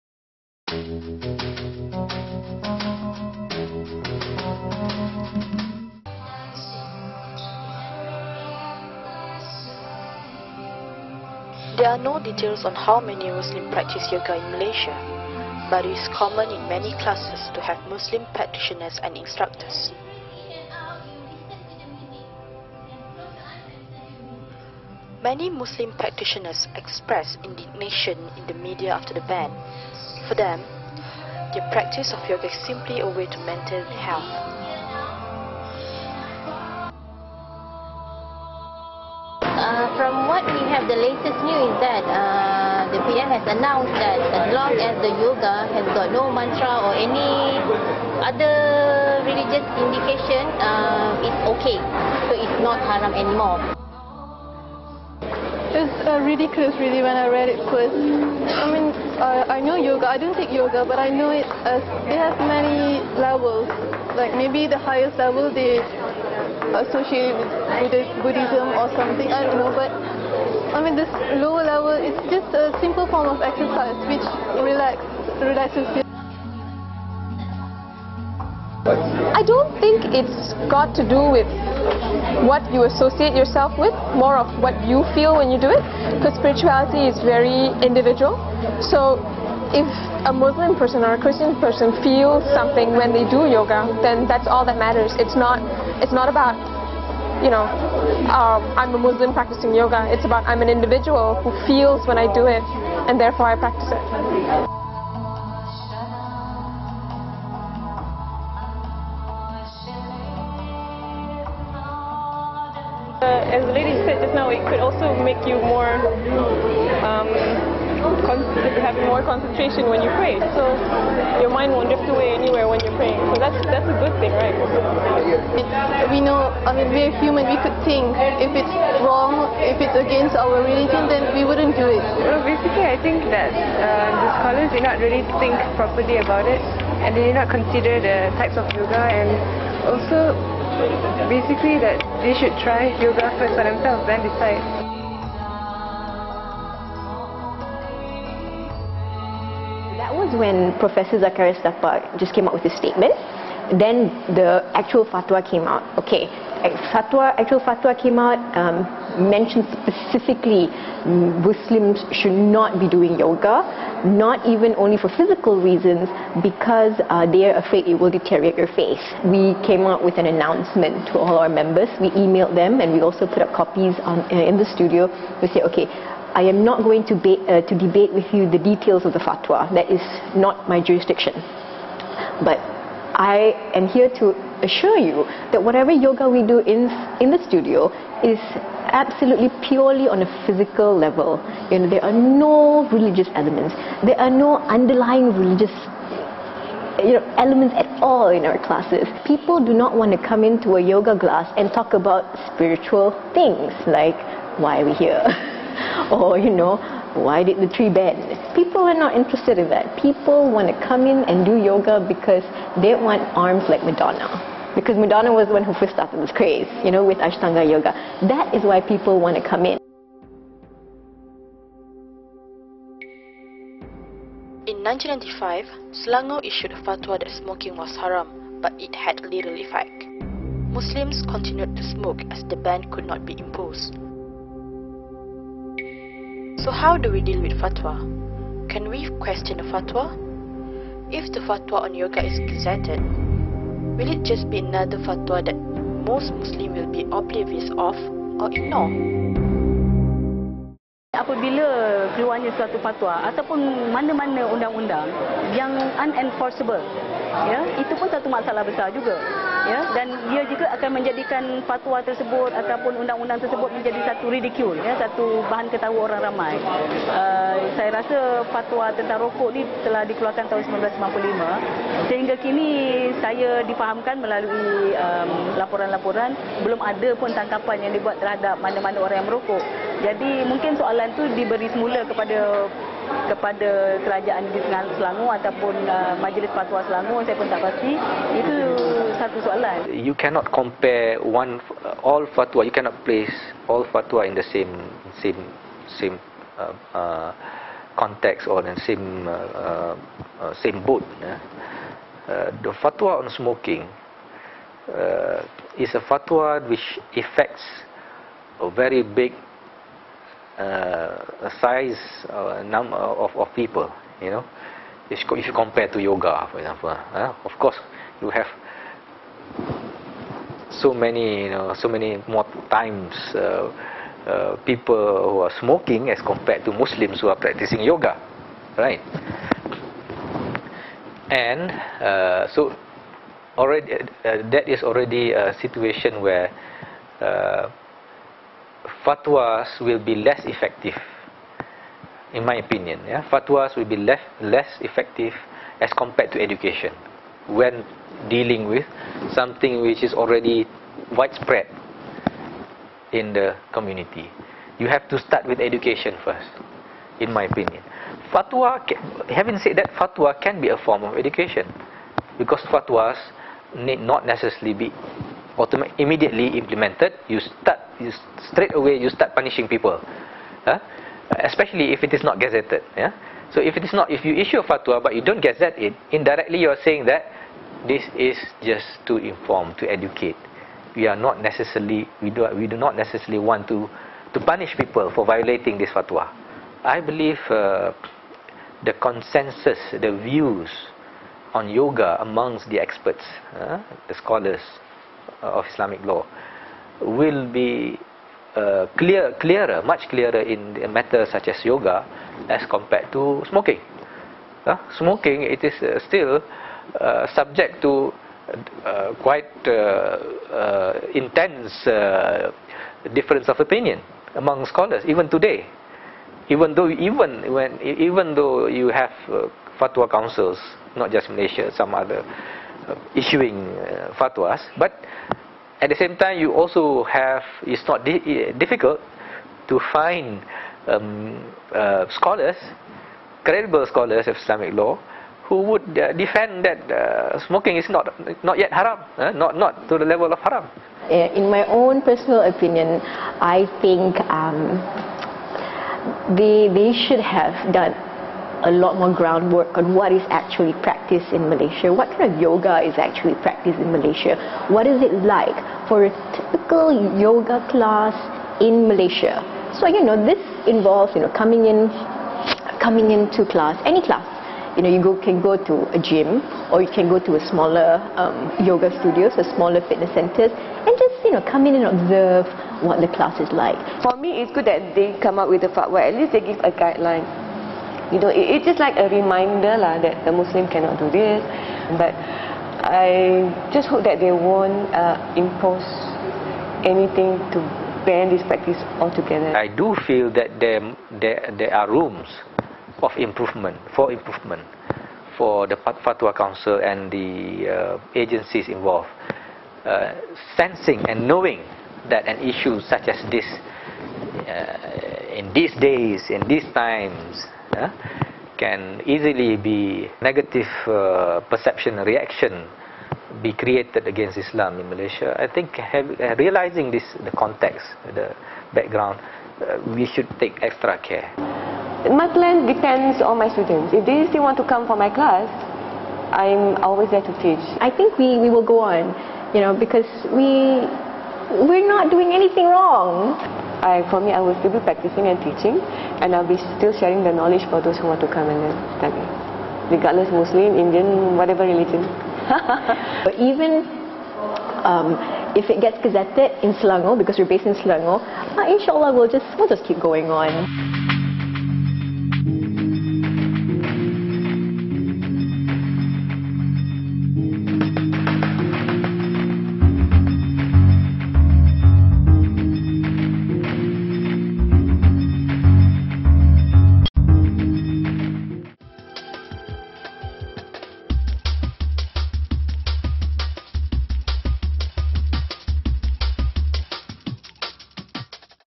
Yoga: Behind the Fatwa - Part II
This is the second and final part of 'Yoga: Behind the Fatwa'.

We continue the discussion on the Yoga Fatwa and ask trainers and practitioners what they are going to do when the Fatwa becomes law for Muslims.

On November 22, 2008, the National Fatwa Council announced that Yoga is 'haram' in Islam and in one fell swoop, Muslims will no longer be allowed to practice it -- once it is gazetted as law.

Yoga attracted what is arguably, unwarranted attention from no less than the NFC as a result of an opinion from an Islamic scholar that Yoga, could be 'haram' in Islam because it could cause Muslims to deviate from his or her faith.

This decision drew criticisms from Muslim and non-Muslim practitioners who have felt real physical and mental benefits of Yoga through its physical exercise and meditative elements.

The Fatwa or religious decree once gazetted, will become enforceable and challenges the rights of individuals of a particular faith from practising what is essentially an ancient form of exercise.

The reason for the ban -- only that Yoga has Hindu roots.

Malaysiakini talks to Yoga instructors and practitioners on their views towards the ban.

We also talked to Norhayati Kaprawi of Sisters-In-Islam and Yusri Mohamad of Angkatan Belia Islam Malaysia (ABIM) for their views on the Fatwa.

Producers: Lydia Azizan & Azreen Madzlan
Editor: Lydia Azizan
Camera: Shukri Mohamad & Fiqtriey Al Haqimiey